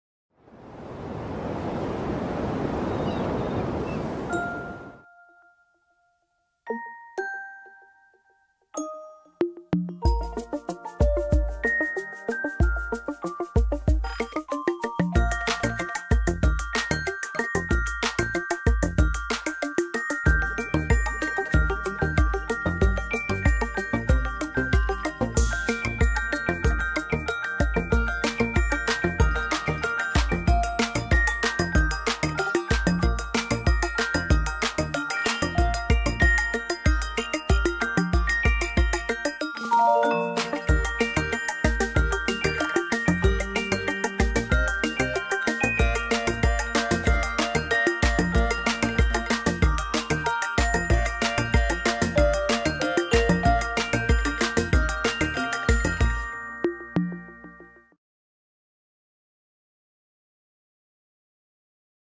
HA035 Inter-Faces : le canapé Mariposa de Barber & Osgerby pour Vitra
Inter-Faces vous présente le canapé Mariposa de Barber & Osberby.
Vitra.
Disponible dans nos magasins à Toulon et Puget-sur-Argens.